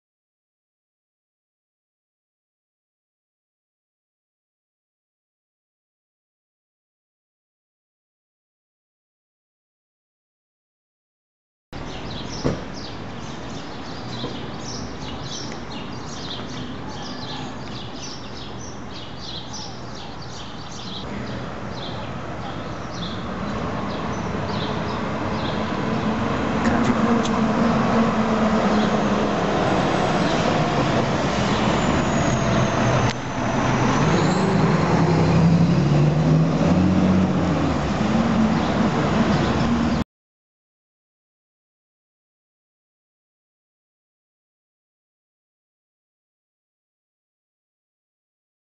Bomberos DF Bomba 0064 + Saludo!
Saludos a Bomberos Benito Juárez Guardia Azul, Roja y Verde, aqui un video de la unidad 0065 que regresa a Benito Juárez luego de unos meses descompuesta. es una unidad Spartan Gladiator con el numeral 0064 del Heroico Cuerpo de Bomberos del Distrito Federal, un saludo especial a la guardia Azul
México DF
[C] 2015